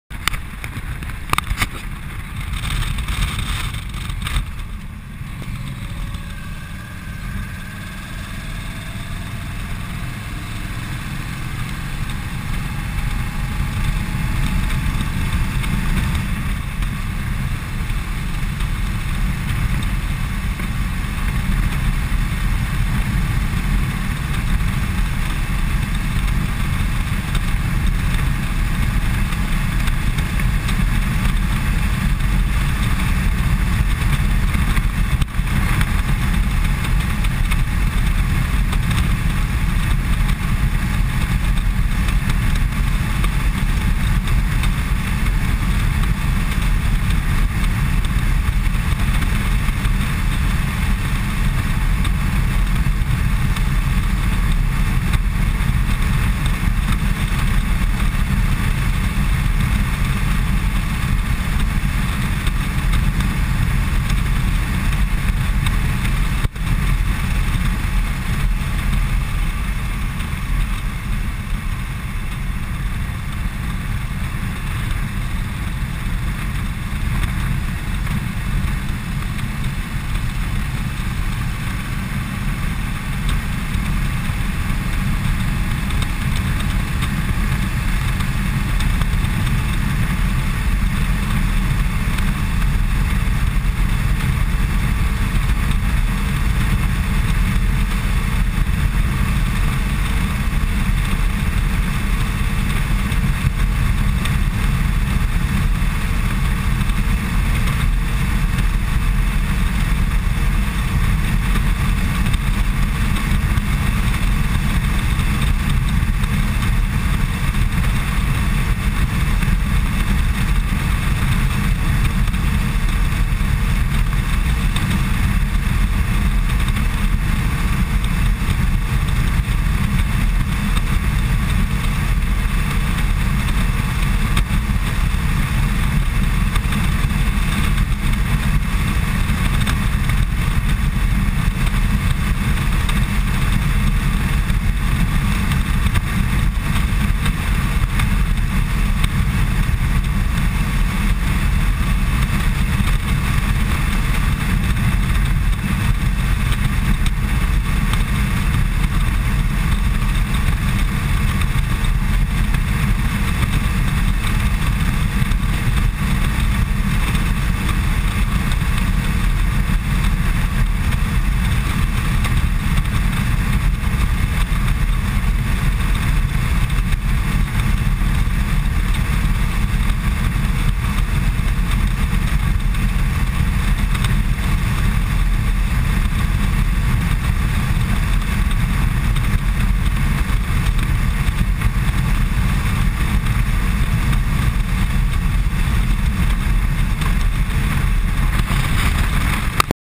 LLegando a Freire.MP4
Ruta Villarrica a Freire..las vistas son antes de llegar a Freire.